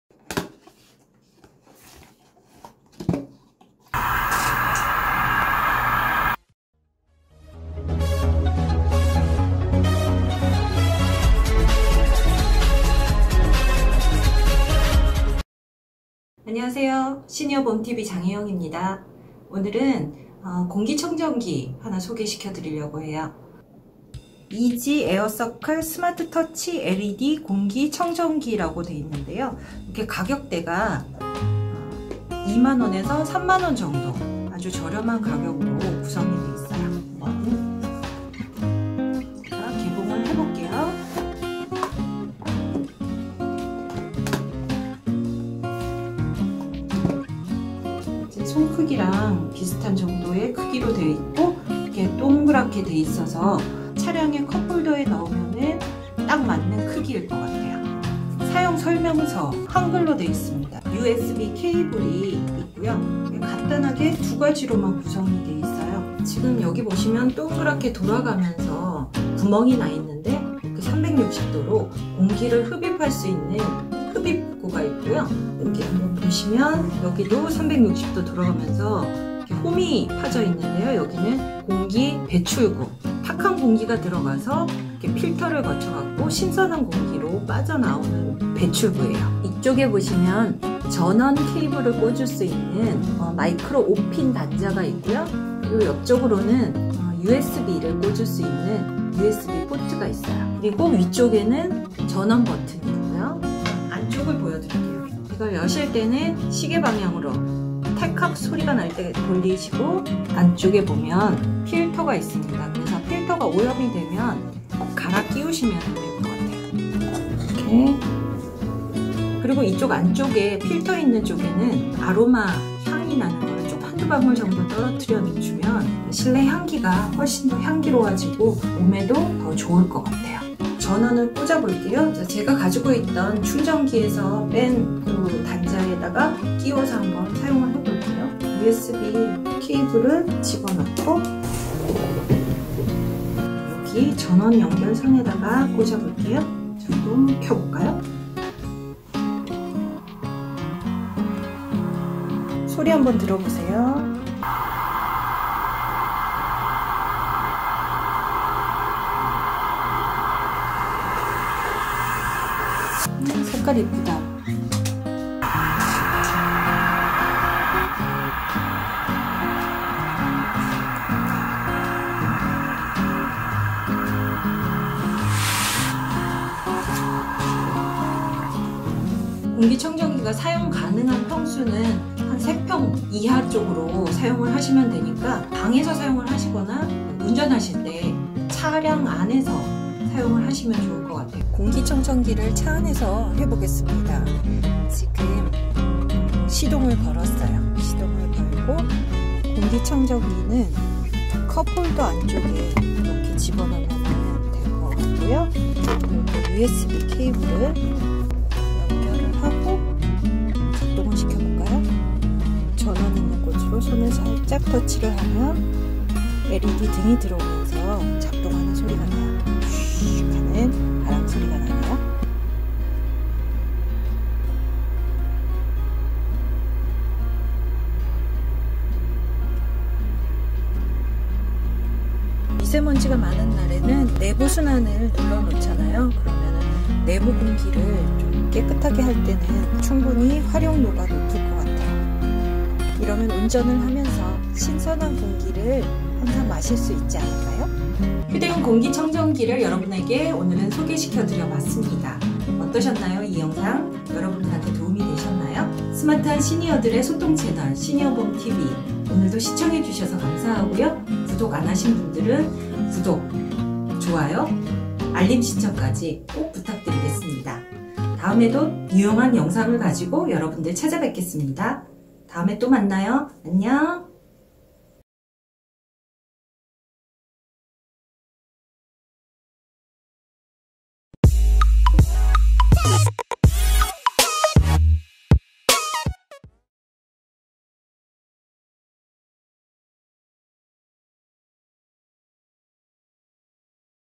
휴대용 미니 공기청정기로 내방,내사무실,내차량 미세먼지 제거, ez air led공기청정기 개봉기/시니어봄tv
휴대용미니공기청정기를 사은품으로 받았습니다. 개봉하고 사용해 볼께요.